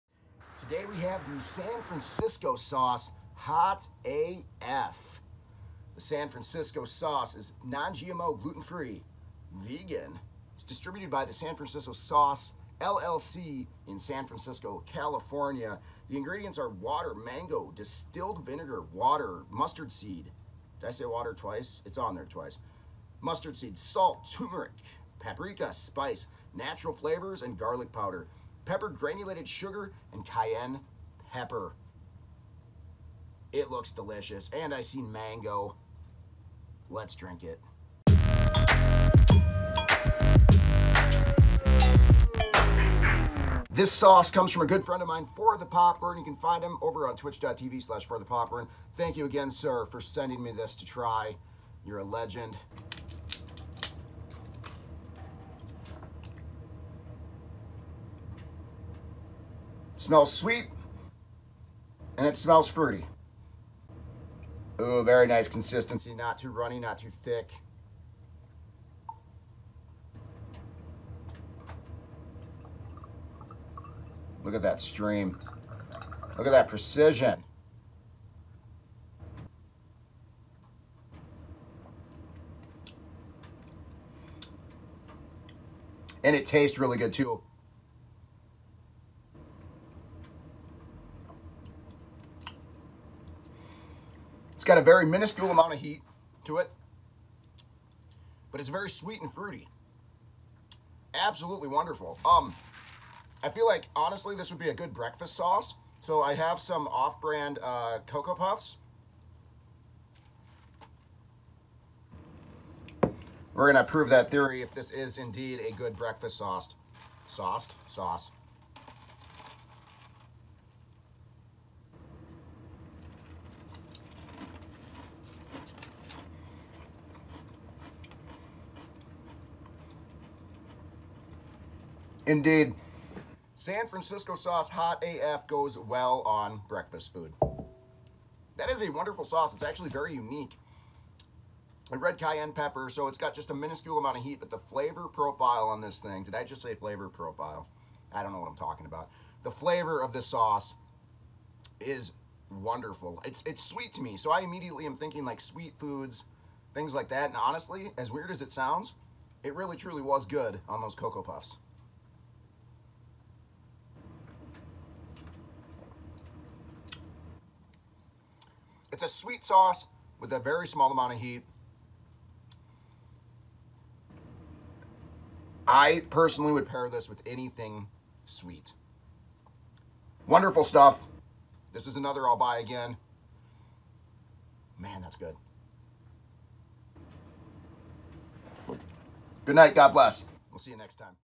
The San Francisco Sause- Hot AF
A sweet fruity sauce that would go great on anything!  Shout out to ForThePopcorn for sending me this wonderful sauce to sample!